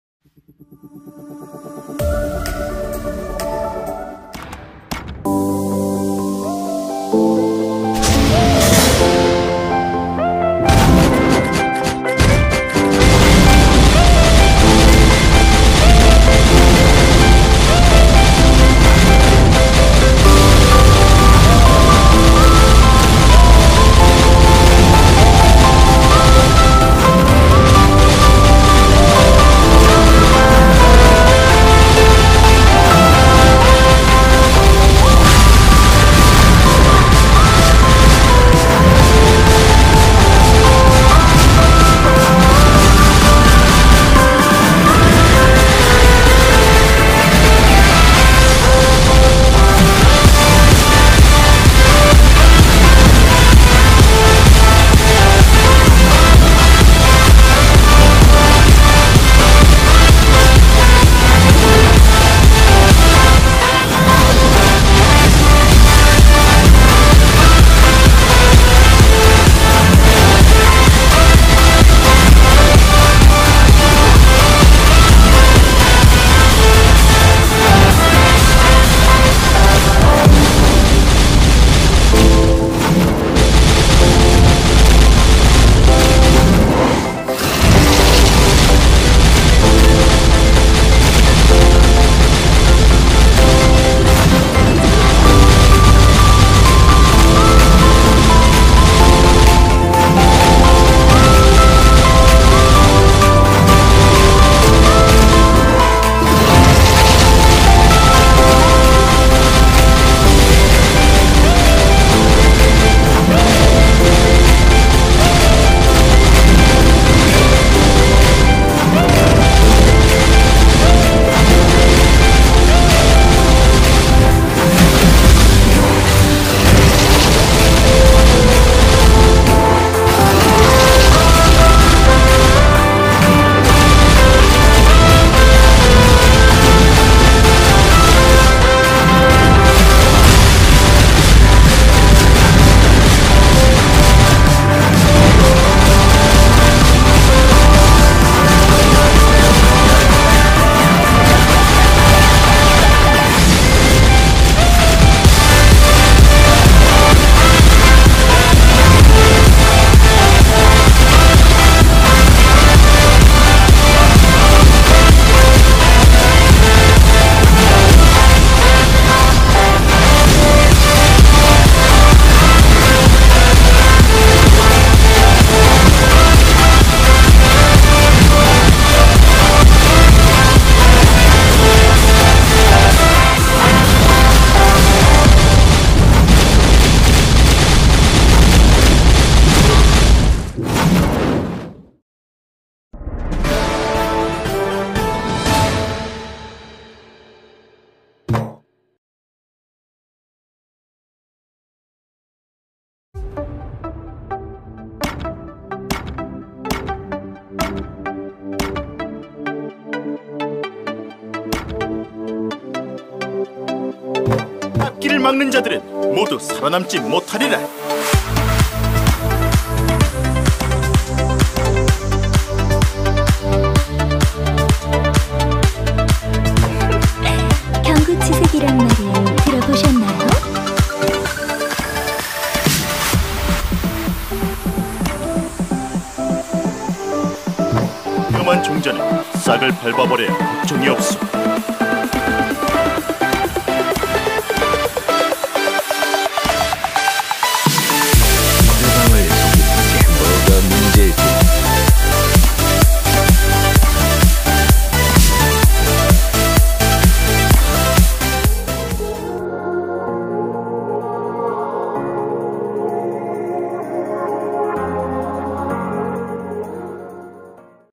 [토벌전] - 조운⚔ 공략 #4
00:00 오프닝
00:05 Boss 조운
03:20 스팩 확인(손견/조비/산초/육손/방통)

패턴 6가지
검기가 3번을 3개, 2개, 5개 이런 순서로 나옵니다.
대부분 지그재그로 피하는데, 5번 날아오는 검기★는 패턴 익혀두세요.
1. 푸른 검기(325, 옆, 중앙, 옆으로 피함)
2. 푸른 검기(234, 중앙, 옆, 중앙으로 피함)
3. 푸른 검기★(32333, 옆, 중앙, 옆, 옆, 옆으로 피함)
※ 공격력 증가(푸른 검기 → 붉은 검기)
4. 붉은 검기(325, 옆, 중앙, 옆으로 피함)
5. 붉은 검기(234, 중앙, 옆, 중앙으로 피함)
6. 붉은 검기★(33323, 옆, 옆, 옆, 중앙, 옆으로 피함)